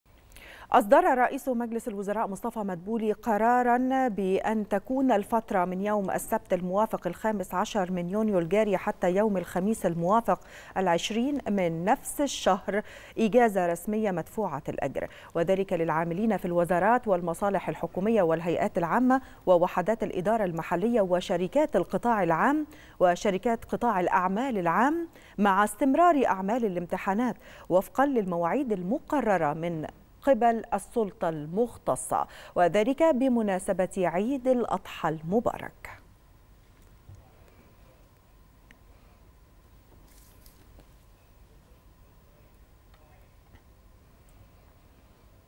الحكومة: إجازة عيد الأضحى من السبت 15 يونيو حتى الخميس 20 يونيو
نشرة الثالثة | الحكومة: إجازة عيد الأضحى من السبت 15 يونيو حتى الخميس 20 يونيو